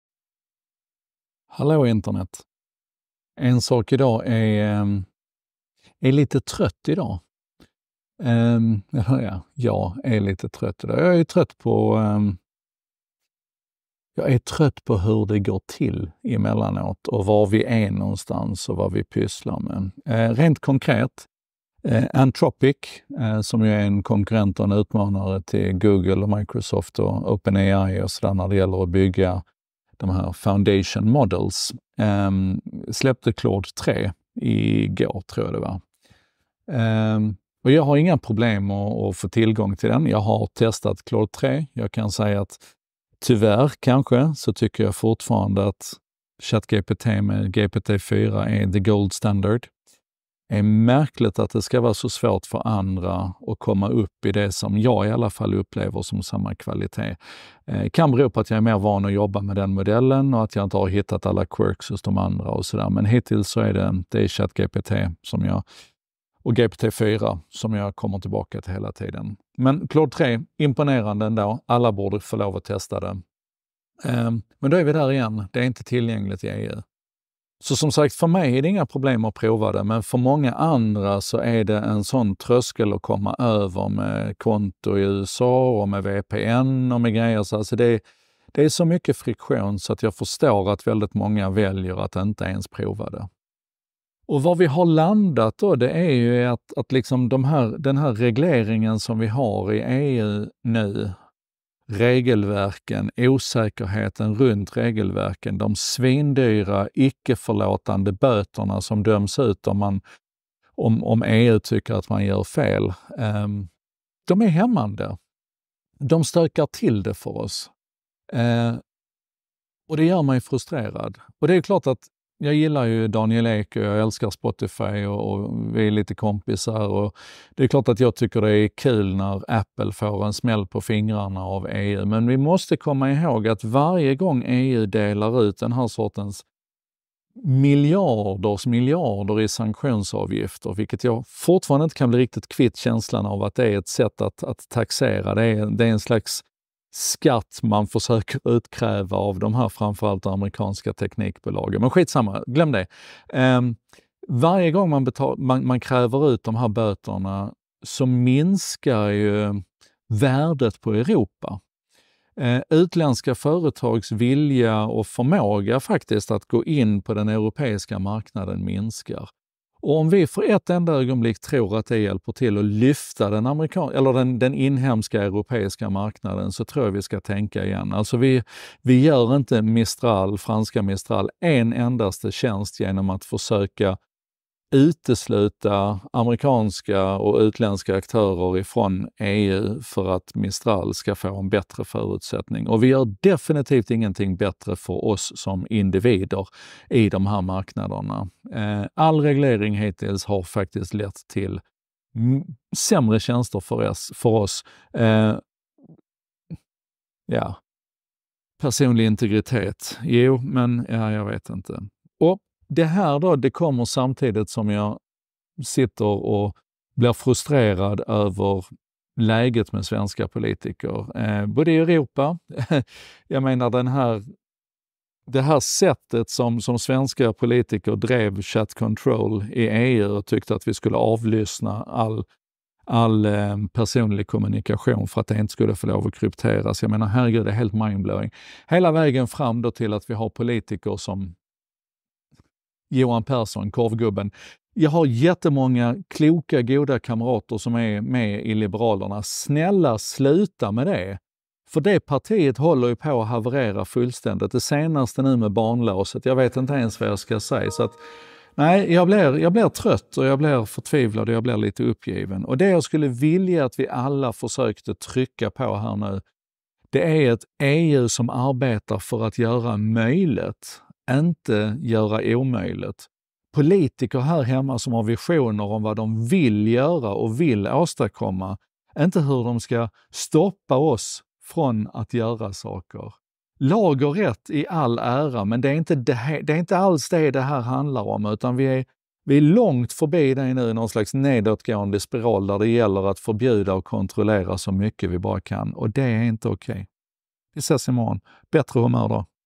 ensakidag 490 – Claude 3 och EU:s klåfingrighet ⚖️💰
… (på marginalen) om Anthropic och deras senaste skapelse Claude 3, en aktiv spelare i AI-racet mot jättar som Google och OpenAI. Mitt i detta AI-brus står EU:s stränga regelverk som en tröskel mot innovation och tillgänglighet. Det är inte svårt för mig, som nörd, att hitta sätt att testa nymodigheten – men den förblir den låst bortom EU:s gränser för många. Detta väcker frågor om balansen mellan reglering och innovation, om hur EU:s nuvarande ställningstagande kan hämma både utveckling och tillgänglighet. Det är dags att reflektera över hur vi vill att vår digitala framtid ska se ut och agera därefter. Borde vi inte önska oss politiker och institutioner som jobbar för att göra omöjligt, snarare än att göra omöjligt?
Googla: Anthropic Claude EU